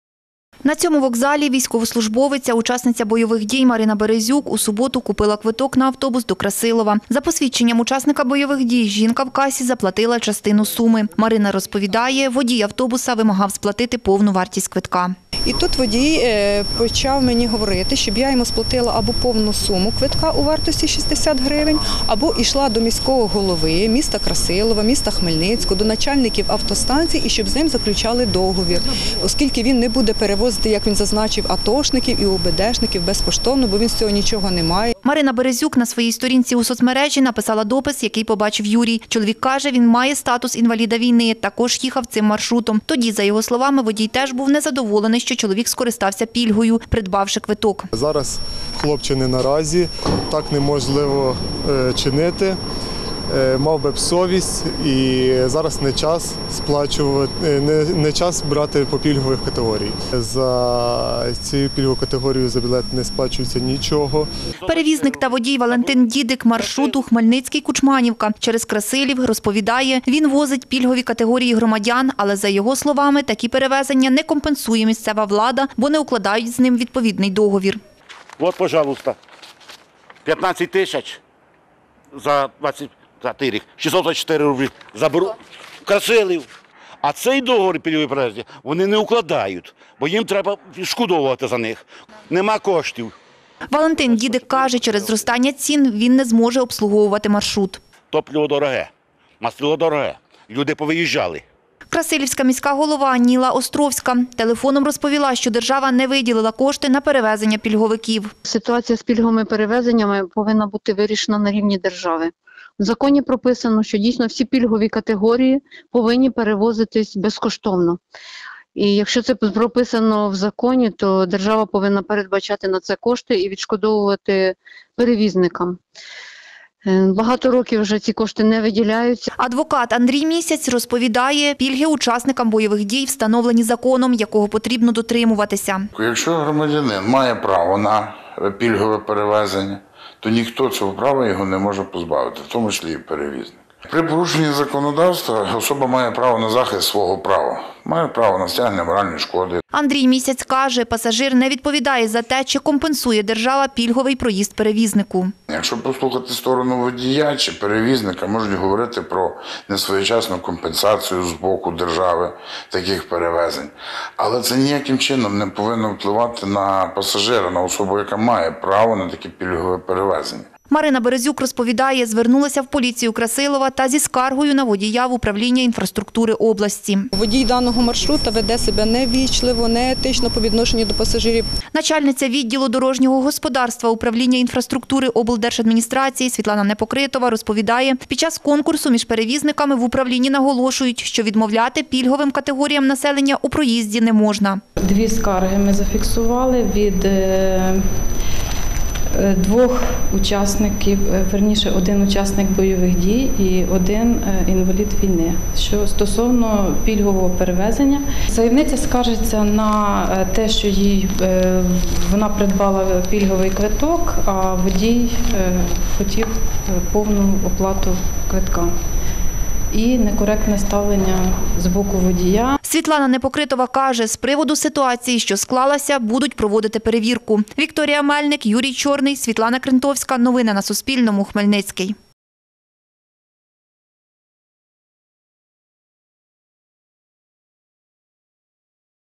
У Хмельницькому водій автобуса відмовляє у пільговому проїзді військовим
Зі скаргою на перевізника маршруту Хмельницький –  Кучманівка через Красилів звернулася військовослужбовиця, учасниця бойових дій в Управління інфраструктури Хмельницької області. Жінка скаржиться, бо водій цього маршруту вимагав від неї сплатити повну вартість квитка, який вона купила за пільговою ціною в касі, пред’явивши посвідчення учасника бойових дій. За словами жінки, водій відмовлявся возити ветеранів війни за пільгами, бо йому держава не компенсує їх перевезення. Кореспондентка Суспільного поспілкувалася з обома сторонами цього конфлікту.